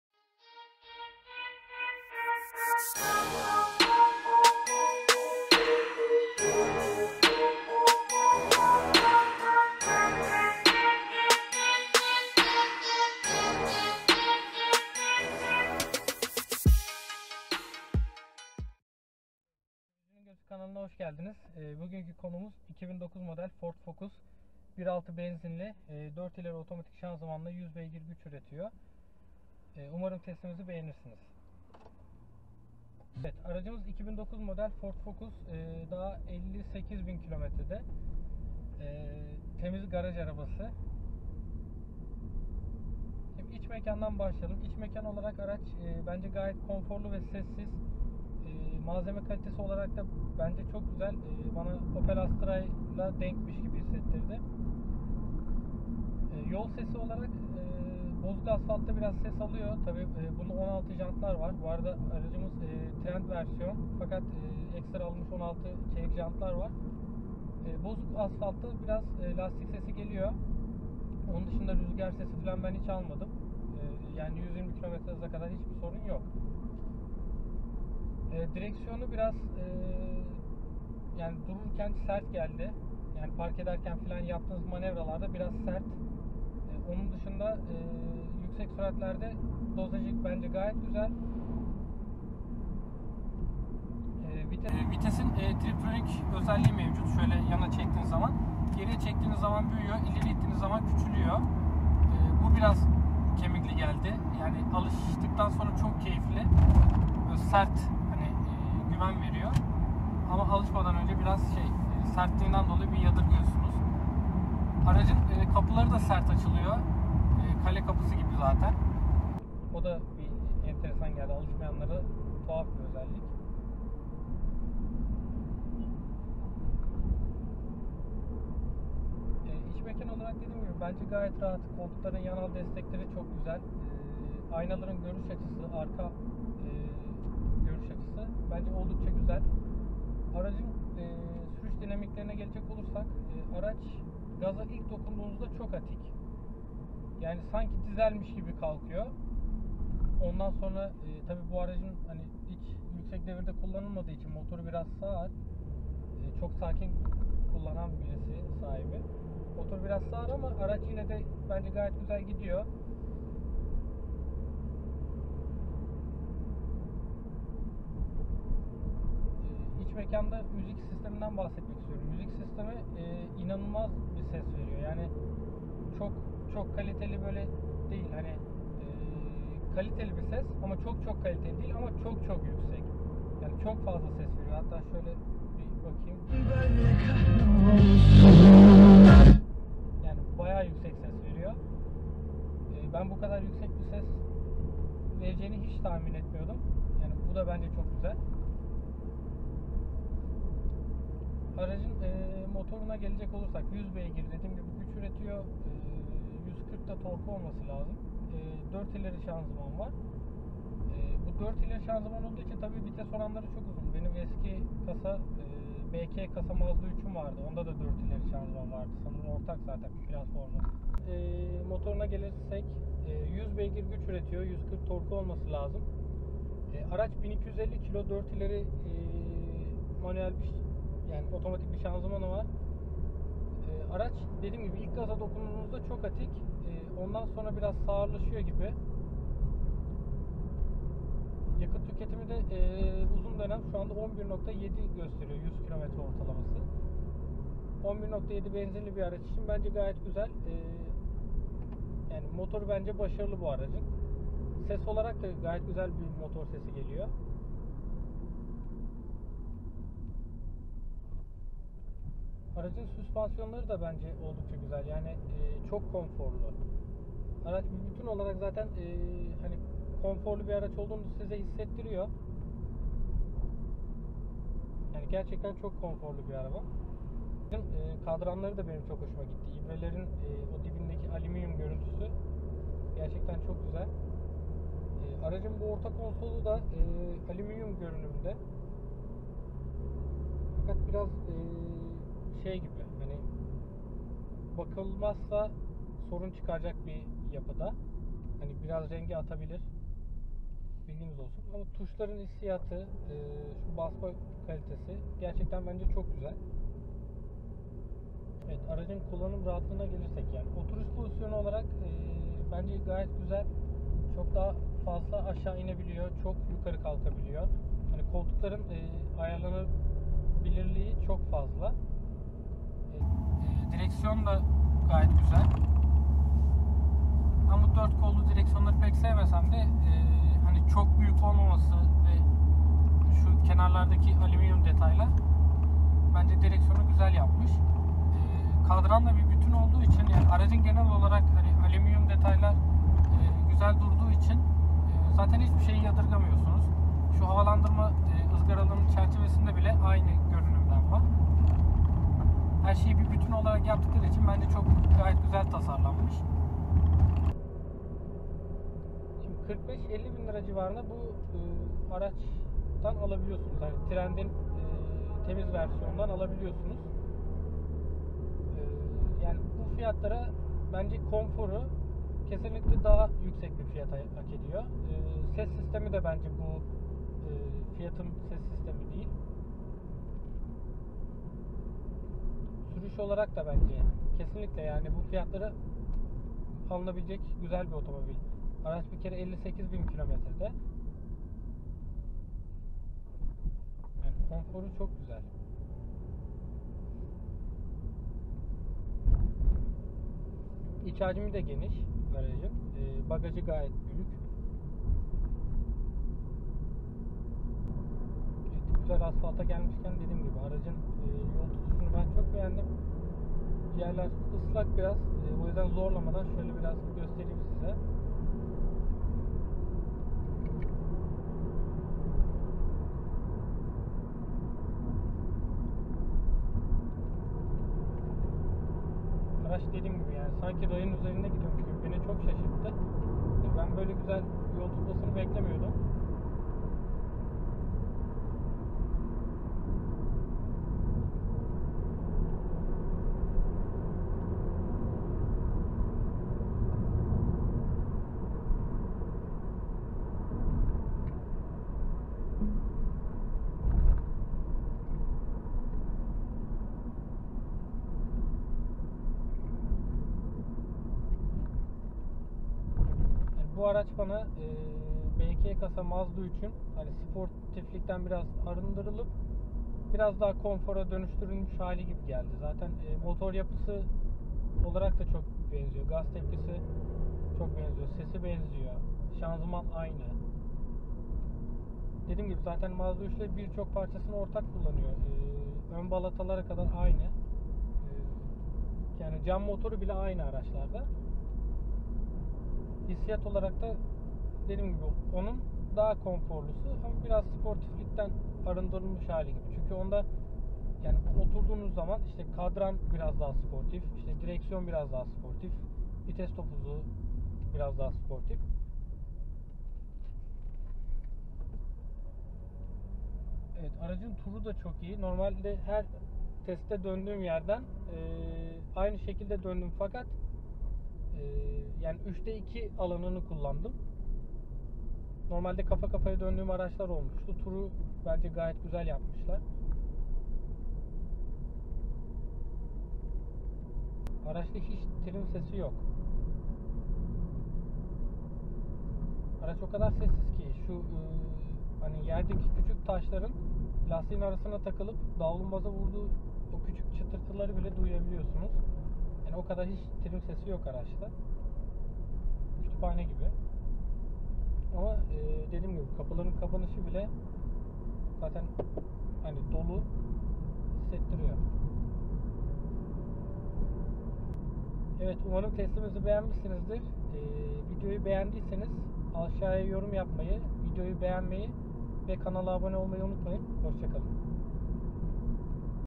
Ford Focus 1.6 Benzinli Otomatik Test Sürüşü / Yakıtı nasıl ? / Sürücünün Gözü
Herkese merhaba Sürücünün Gözü kanalına hoş geldiniz.

Ford Focus 1.6 Benzinli Otomatik ile gerçekleştirdiğimiz bu test sürüşümüzde aracın tüm noktalarına değinmeye çalıştık.

Ford Focus 1.6 Benzinli Otomatik iç mekan olarak kalite algısı oldukca yüksek.İçeride kullanılan alüminyum detaylar gerçekten göz dolduruyor.Özellikle kadranlarda kullanılmış detaylar insanı memnun ediyor.

Ford Focus 1.6 Benzinli Otomatik konfor olarak 16 jantlarla sunduğu konfor gerçekten güzel.Ses sistemi olarakta bizi şaşırtan araç hiçte dışardan göründüğü gibi değil.

Ford Focus 1.6 Benzinli Otomatik aracımız şanzıman olarak 4 ileri tork konvektörlü şanzıman kullanmakta. Araç 4 ileri olması ve yüksek devir çevirmesi nedeniyle biraz fazla yakmakta. Bunun yanı sıra çıkardığı motor sesi gerçekten çok güzel,

Sonuç olarak Ford Focus 1.6 Benzinli Otomatik kesinlikle alınır.